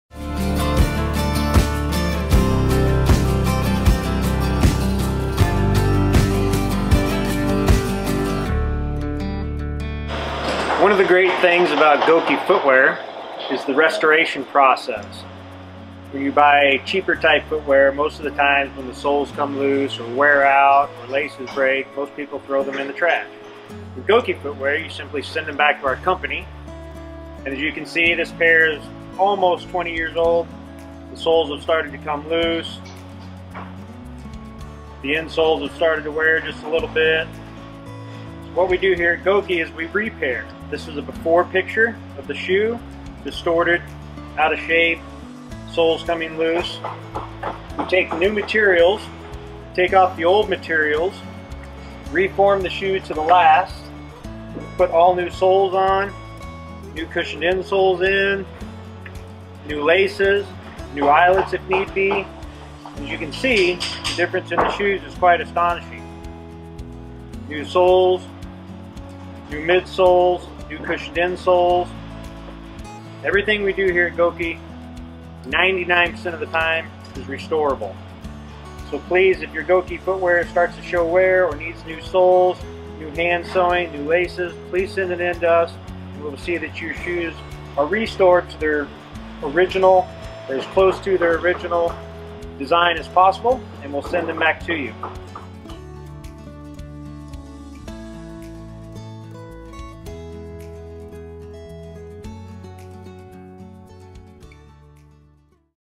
Gokey footwear repair and restoration
Gokey Plant manager Stacy Doss discusses how Gokey shoes and boots needing repair can be restored back to their original condition.